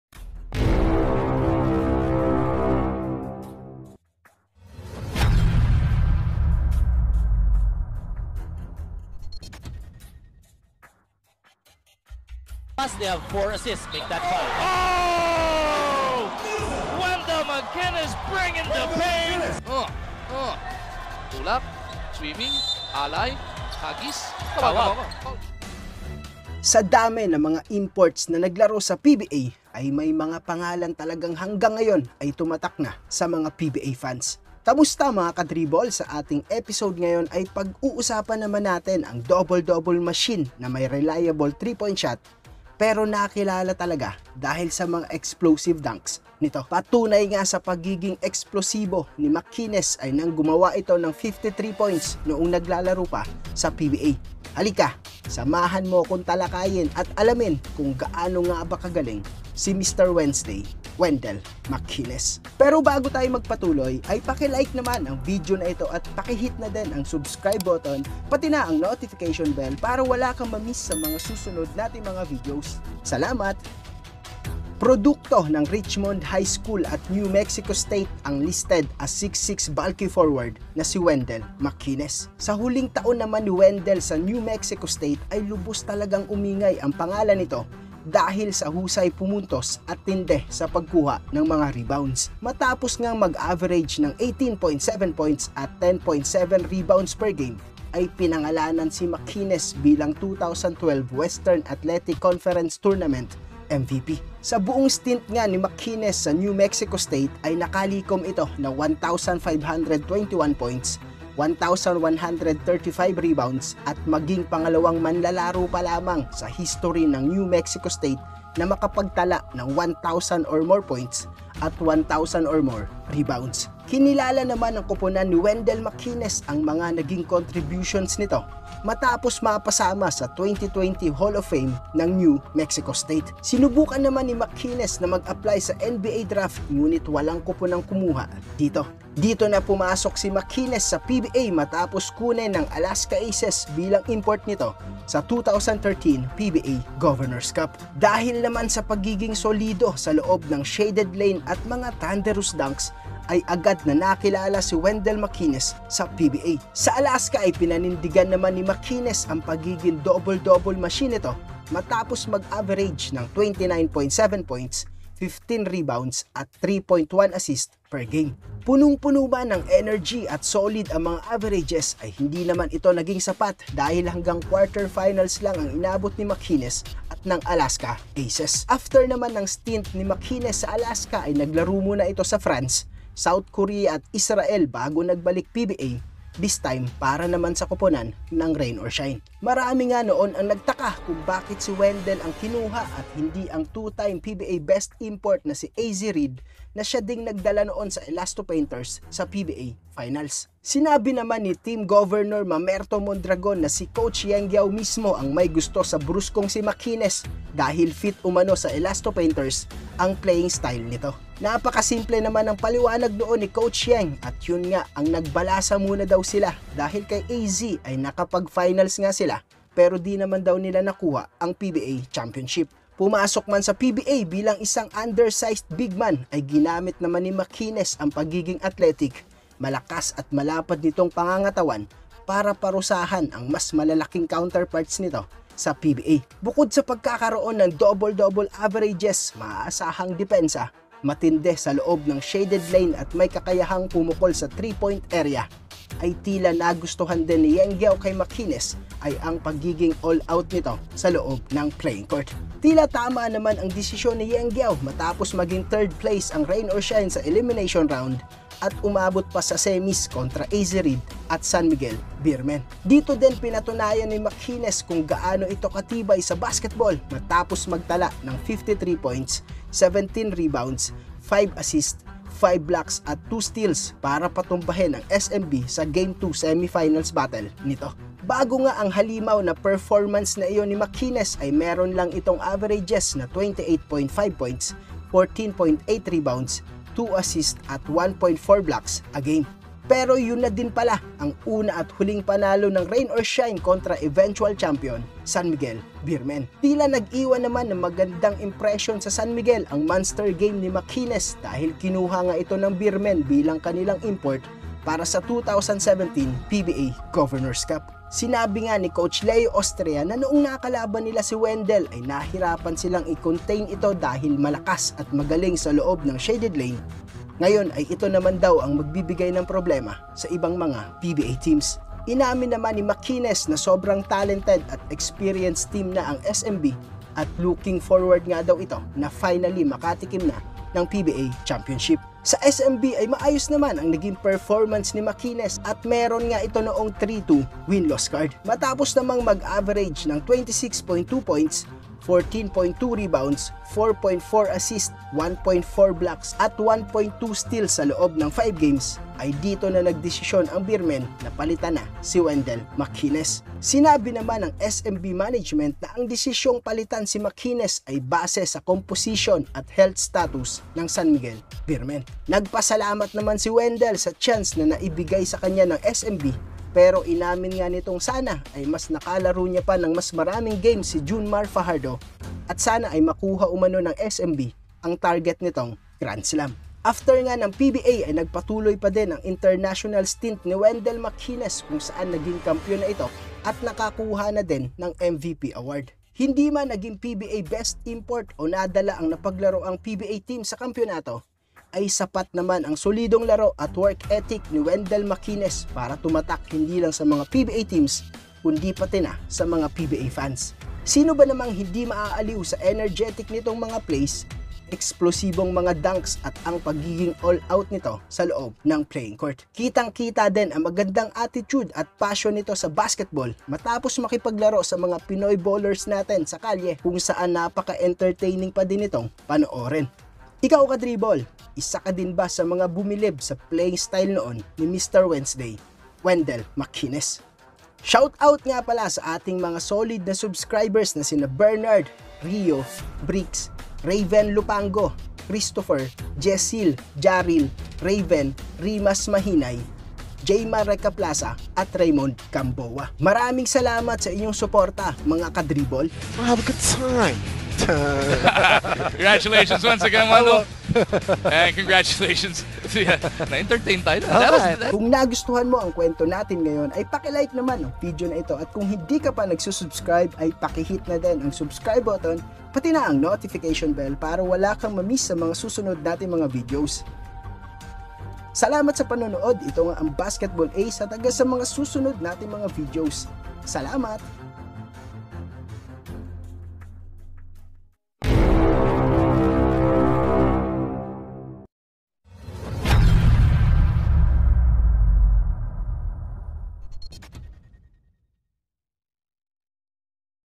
WENDELL MCKINES STORY: EKSPLOSIBONG PBA IMPORT | Kasama sa HALL OF FAME | GAANO BA ITO KAGALING?
Mga ka-dribol! Halika at talakayin natin ang WENDELL MCKINES STORY: EKSPLOSIBONG IMPORT sa PBA | KASAMA SA HALL OF FAME | GAANO BA ITO KAGALING?

Basketball Ace. basketball ace.